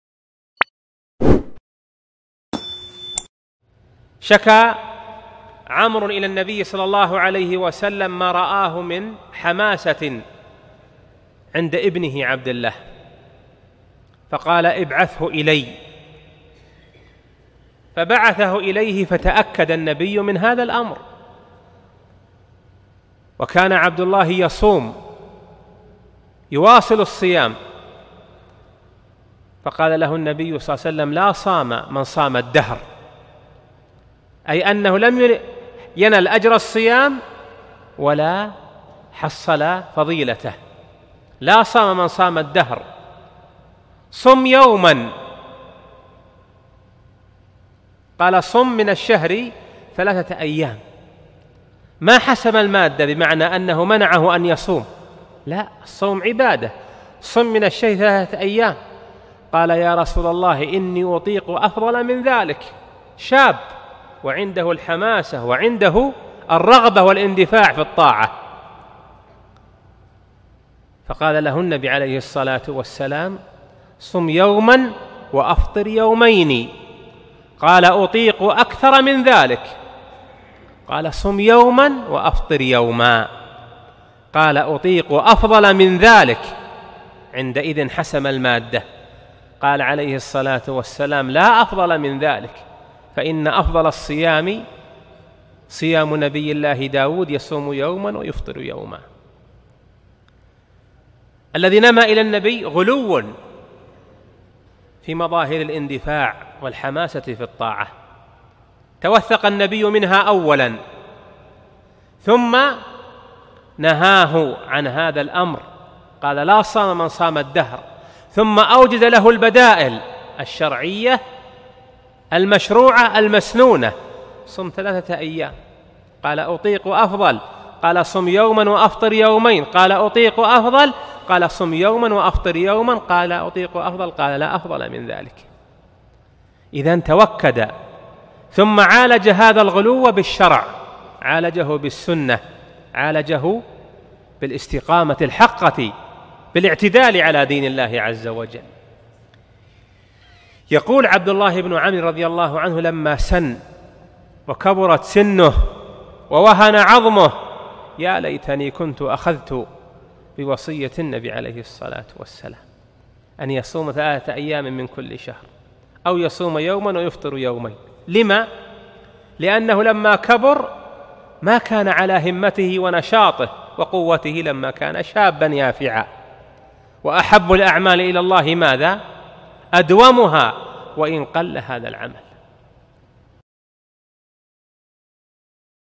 طريقة رسول الله صلى الله عليه وسلم في معالجة الغلو
فضيلة الشيخ الدكتور علي بن عبدالعزيز الشبل